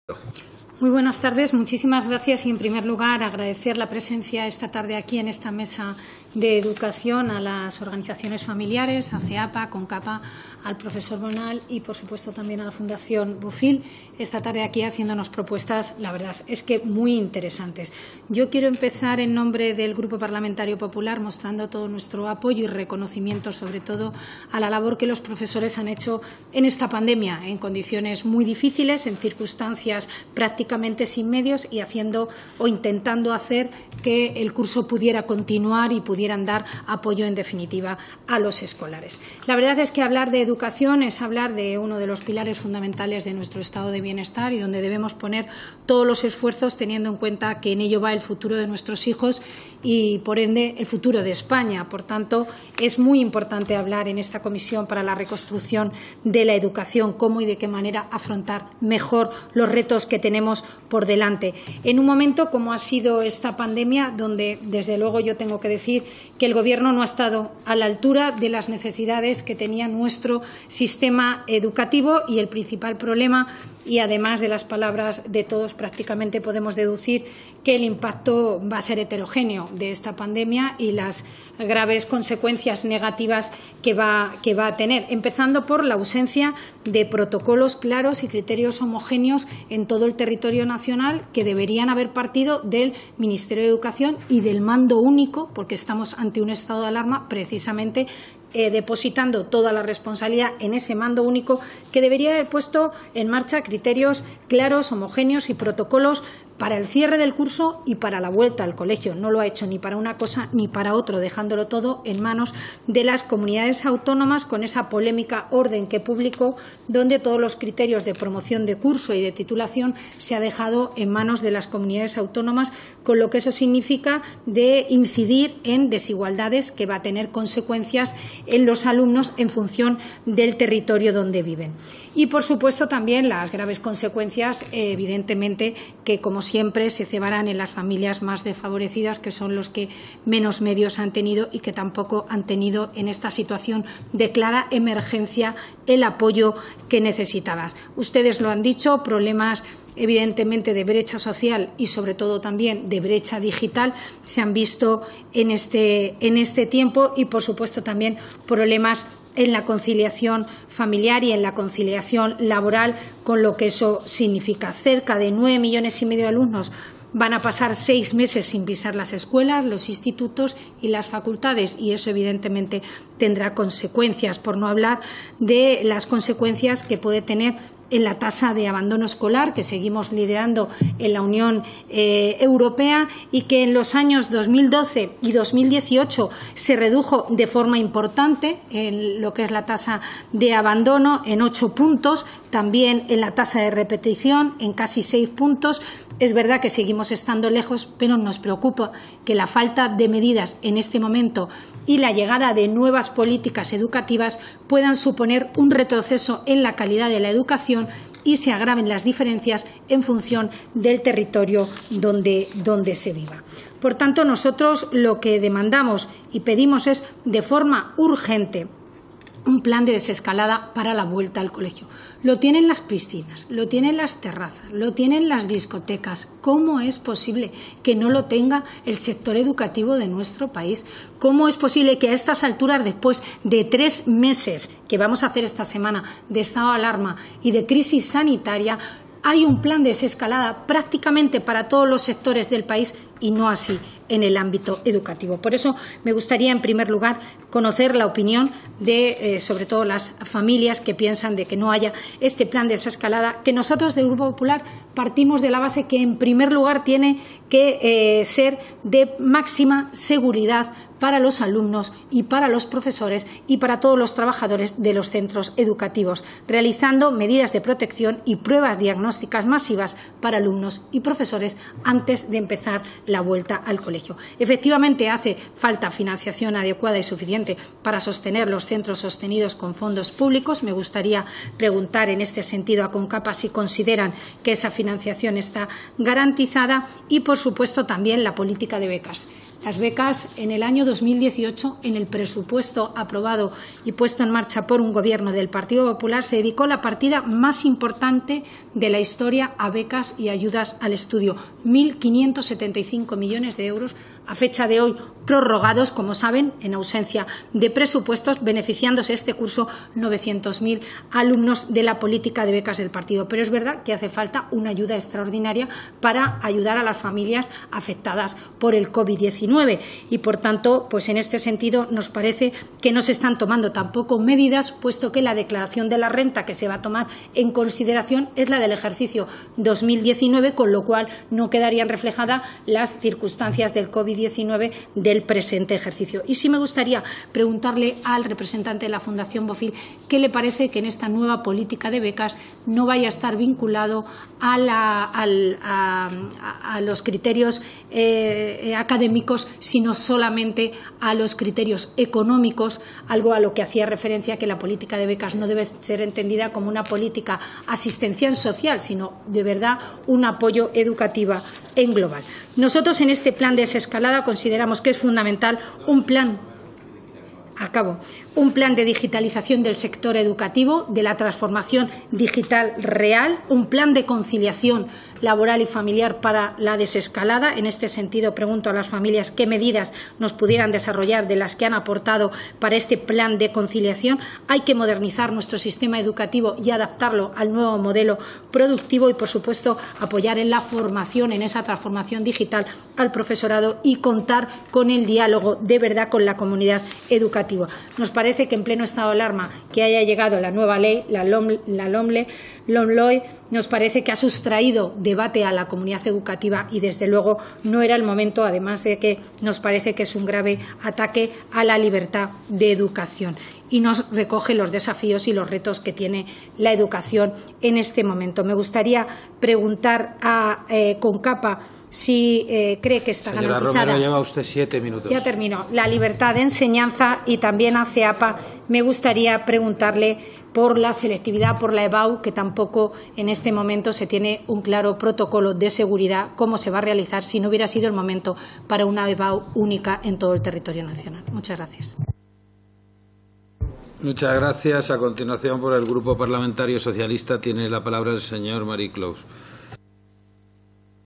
Rosa Romero en el Grupo de Trabajo Políticas Sociales y Sistema de Cuidados
Intervención de Rosa Romero en el Grupo de Trabajo Políticas Sociales y Sistema de Cuidados de la Comisión para la Reconstrucción Social y Económica de España